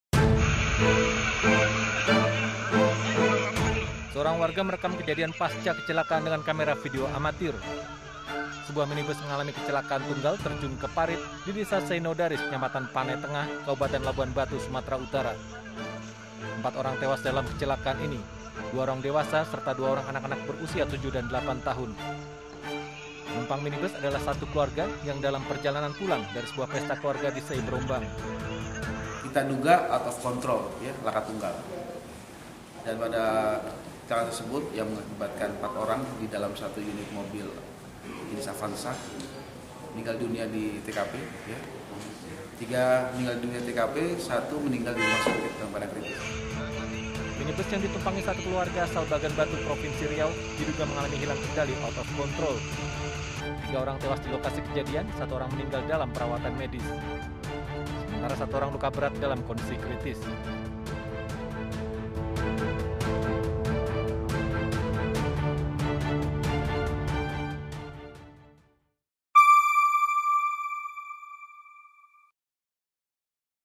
Minibus Terjun ke Parit di Labuhanbatu, 4 Orang Tewas
Sebuah minibus mengalami kecelakaan tunggal hingga terjun ke parit di Labuhanbatu, Sumatera Utara. Minibus yang ditumpangi satu keluarga asal Bagan Batu, Provinsi Riau, diduga mengalami hilang kendali, video selengkapnya di